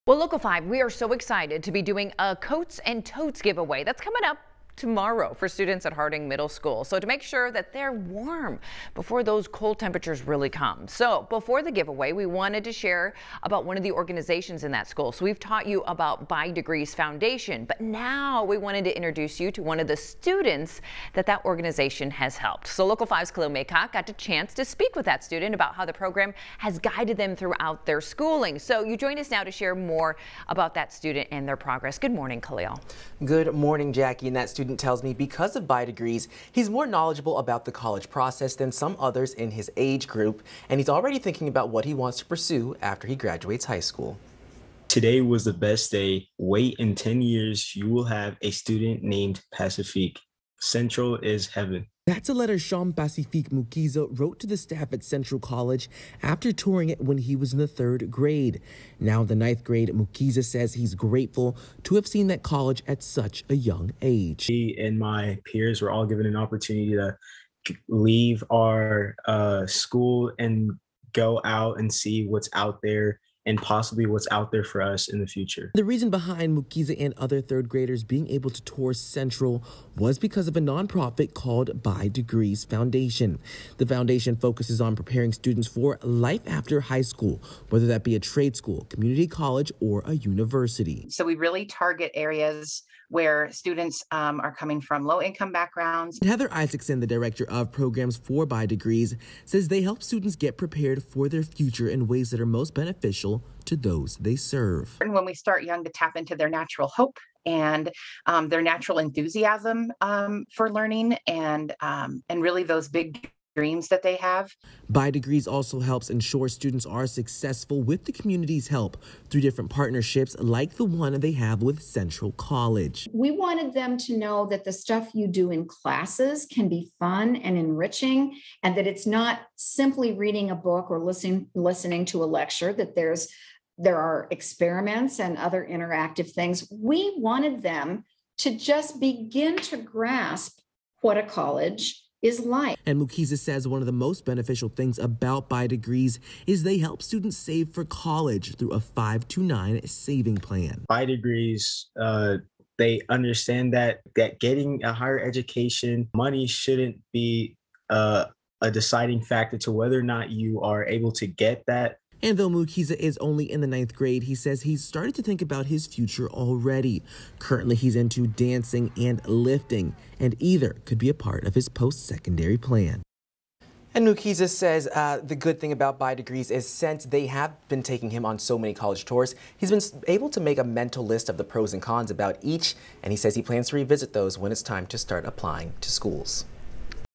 By Degrees Foundation helping students focus on their future
A student in the By Degrees Foundation shares how the program has helped him become knowledgeable about the college process starting at a young age.